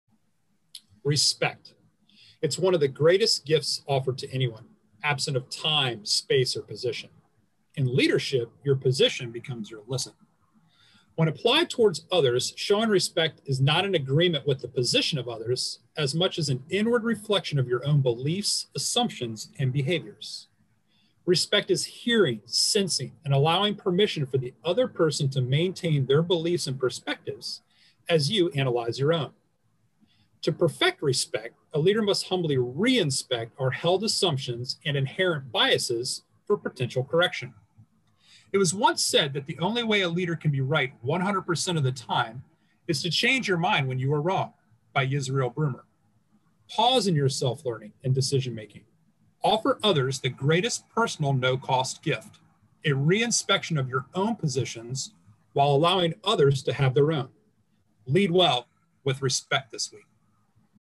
"45 Seconds on Leadership and Ethics" with U.S. Navy Capt. Hunter Bankart, military professor
Week 91 -- Lead well, with respect, this week.